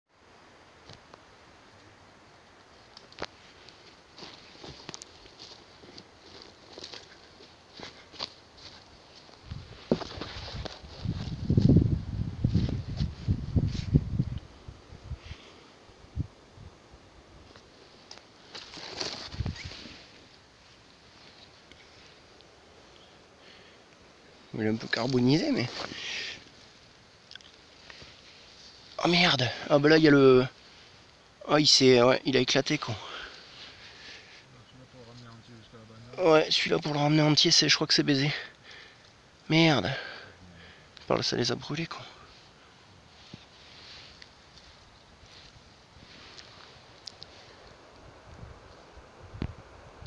Bois de cerf 06/05/17 paire de 10 corps après l'incendie
Recherche des bois de cerf dans le médoc après l'incendie